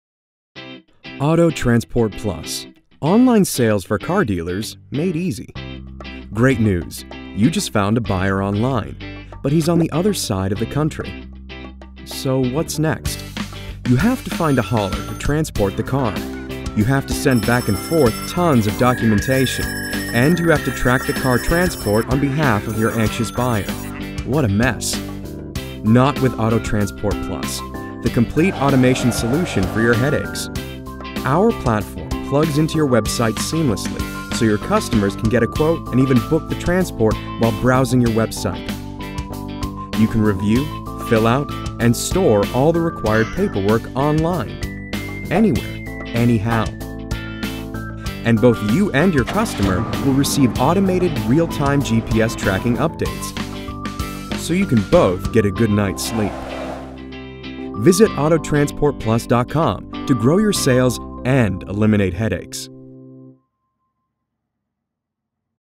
Auto Transport Plus Video #3 HD
Auto Transport Plus offers a unique platform to connect dealers with transportation services all web-based and super-easy to use website. The explainer video highlights how easy to use the online website services.